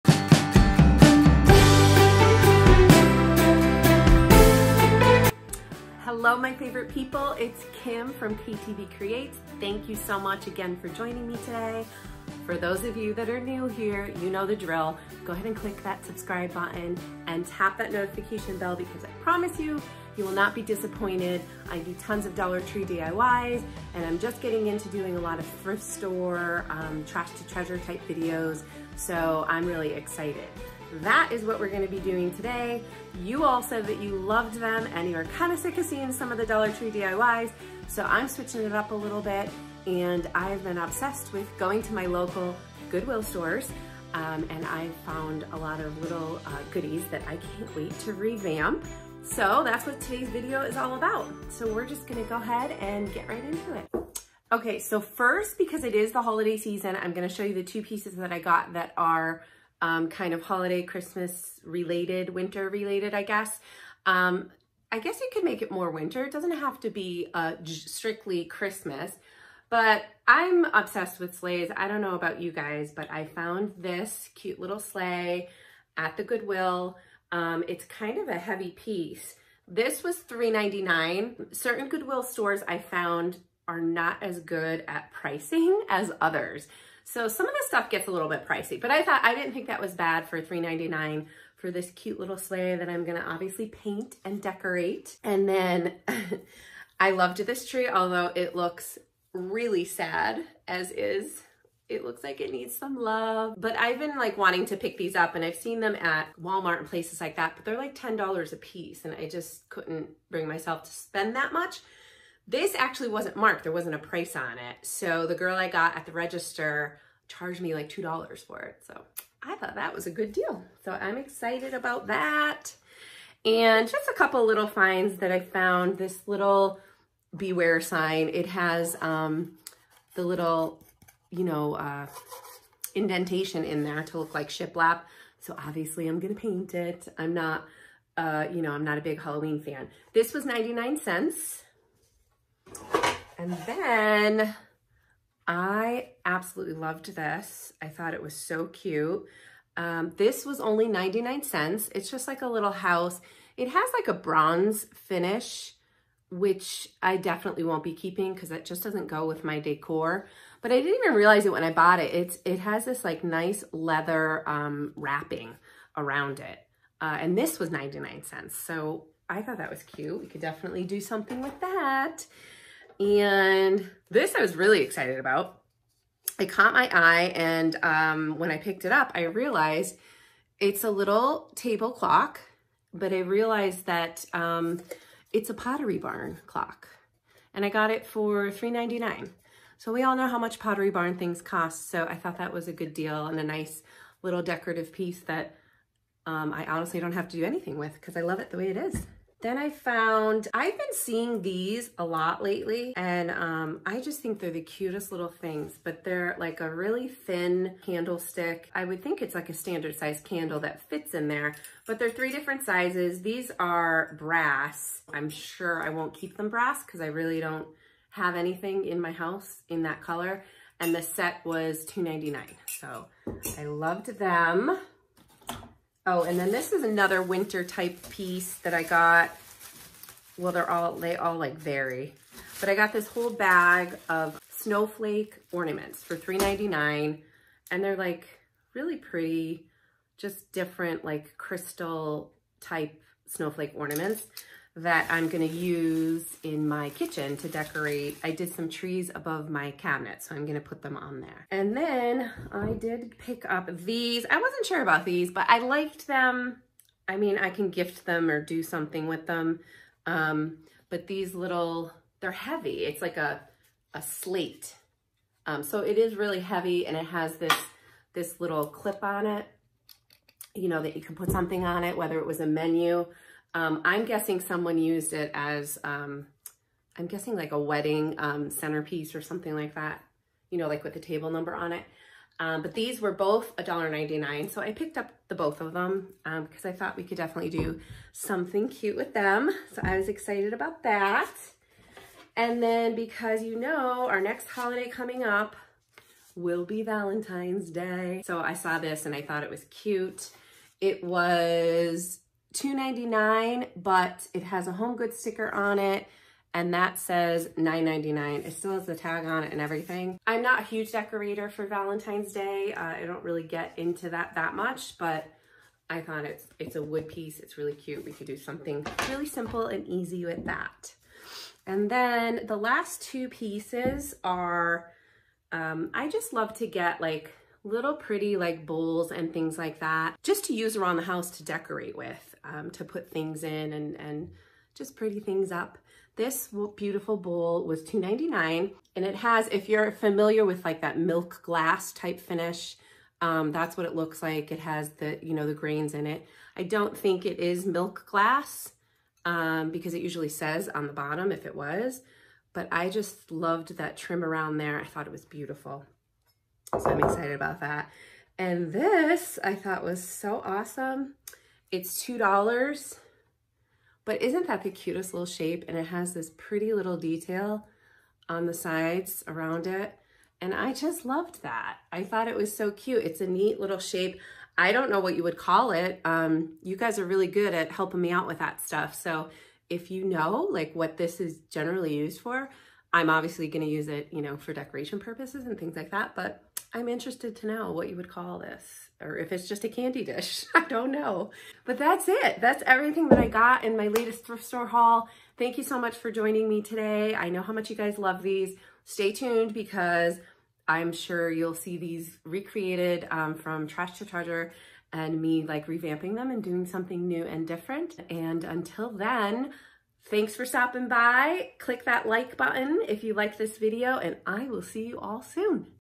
Thrift Store Haul/ Farmhouse Décor/ Goodwill Finds
Join me for my December 2019 Thrift Store Haul, lots of goodies I can't wait to re-create into little treasures! Thanks for stopping by!